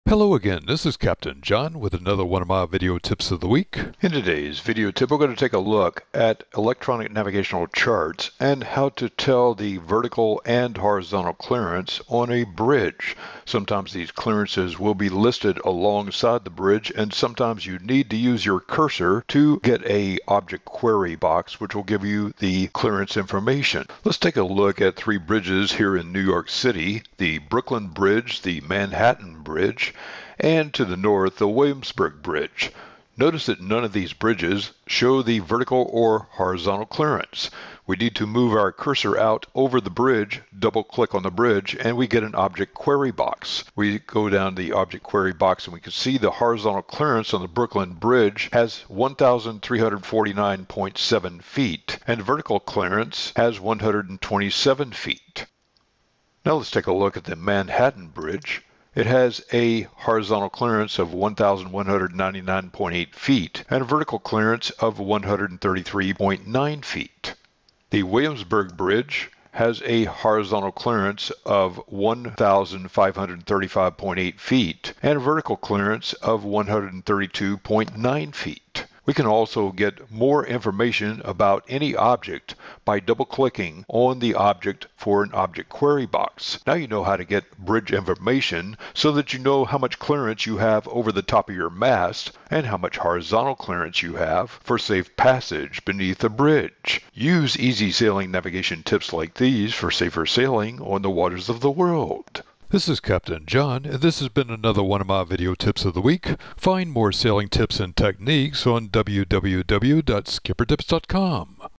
Electronic Navigation Chart Symbol Secrets – Bridge Clearances
Did you know that the latest Electronic Navigational Charts often hide the vertical and horizontal clearance of a bridge? Know this vital info before passing under a bridge to avoid expensive damage to your mast. Watch this sailing video now